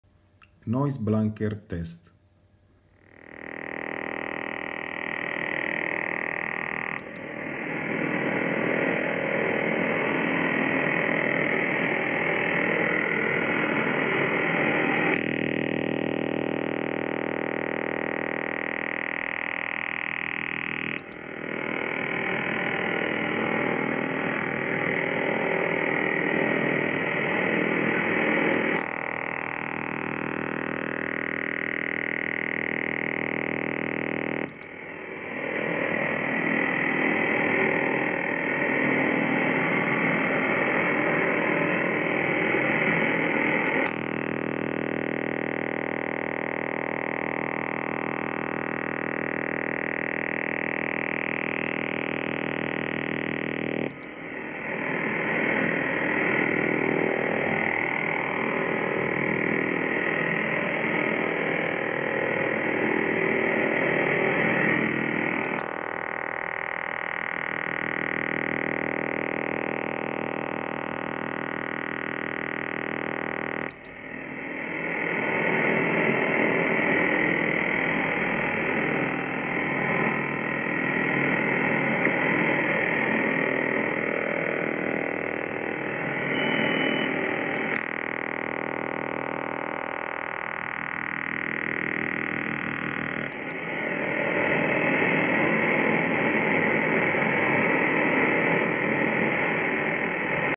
NB test Icom ic-r71 against OTH radar
NB test with my Icom ic-r71 against new Russian OTH radar.